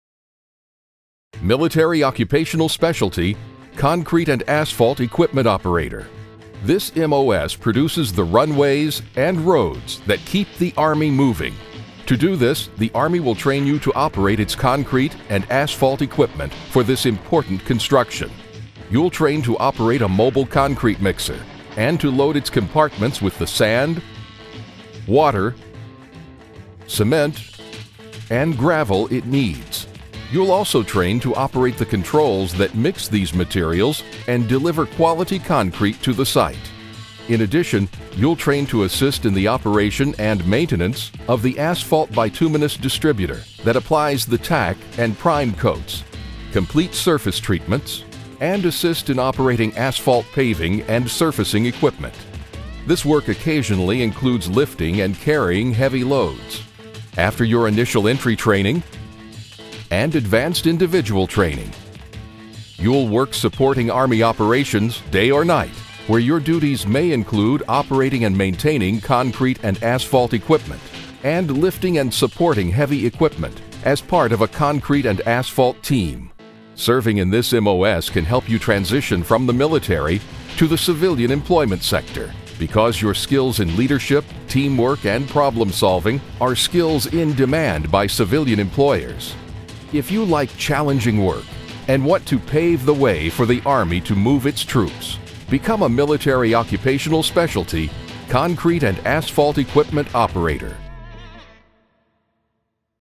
Army Careers 12V - Concrete and Asphalt Equipment Operator
The Army is always completing hundreds of construction projects and Concrete and Asphalt Equipment Operators are integral to this process. Airfields, roads, dams and buildings can't be created without moving tons of earth and building materials. Concrete and Asphalt Equipment Operators use bulldozers, cranes, graders and other heavy equipment to complete these kinds of projects.

Concrete and Asphalt Equipment Operators are responsible for supervising or operating all equipment used in concrete and asphalt production and paving. Some of your duties as a Concrete and Asphalt Equipment Operator may include:

   -Produce concrete with a concrete mobile mixer
   -Assist in the building and operation of asphalt producing plant
   -Operate asphalt distributors, aggregate spreaders and asphalt kettles
   -Assist in the operation of asphalt paving and surfacing equipment
   -Assist in the performance of combat engineer missions